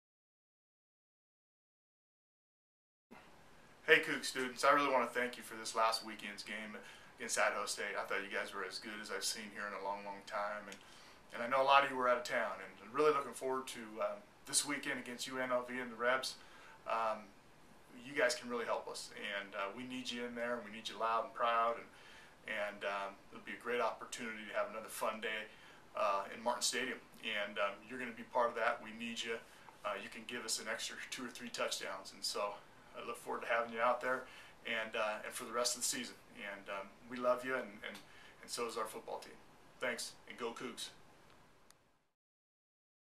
A Message to WSU Students from Head Football Coach Paul Wulff
WSU students! Hear a special message from Head Coach Paul Wulff leading up to the Cougars' game vs. UNLV, Saturday (Sept. 10) at Martin Stadium! Go Cougs!